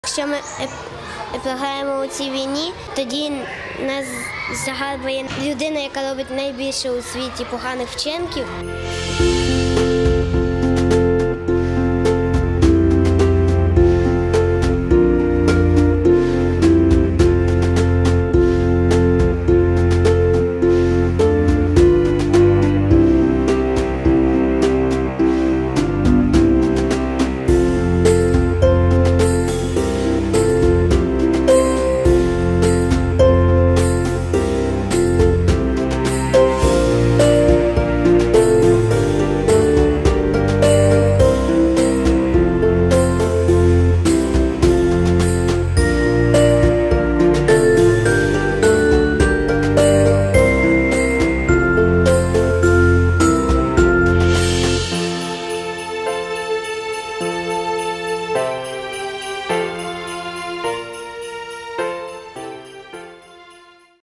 7-річний хлопчик пише казки та підтримує ЗСУ
7-річний Тимофій з Вінниці пише казки, аби допомогти захисникам бити окупантів!

 Зі своєї першої збірки він виручив 15 000 гривень, які передав на потреби воїнів. А у відповідь отримав подарунок, який надихнув його на створення нових казок ❤️

У відео використані матеріали: Телеканал ВІТА, Вінницька міська рада, ГЕРОЙ CAR.